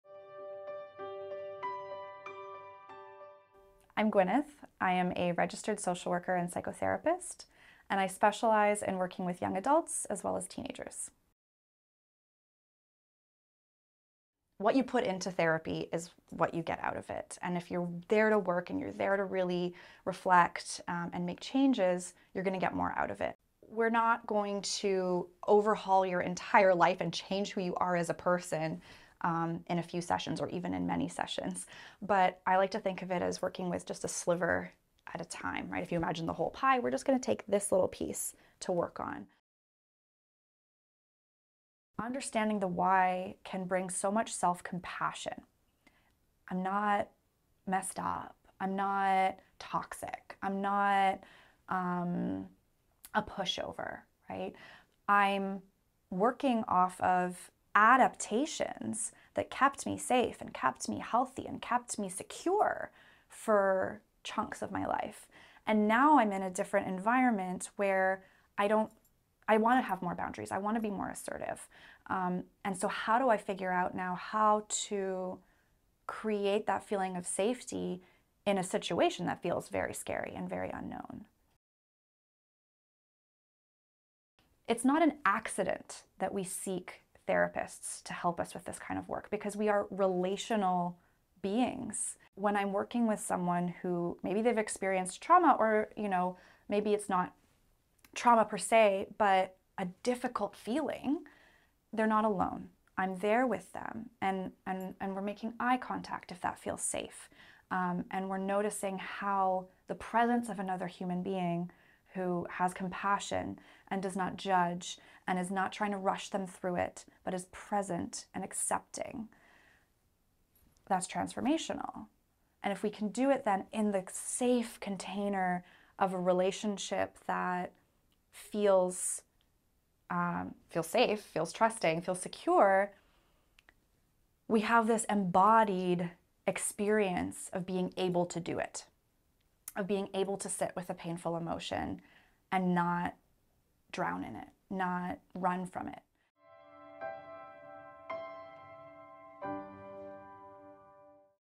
Gwyneth Hodgins, Registered Social Worker | First Session
Gwyneth is a compassionate and non-judgmental therapist. She especially likes working with teens and young adults on issues around identity, self-esteem, stress, and relationships. Going to therapy can be scary, but Gwyneth tells her clients to focus on one part of their life first, rather than trying to tackle everything all at once. She is there to create a safe space, to listen to them, and provide support. When not at work, Gwyneth likes doing yoga and spending time with her dog.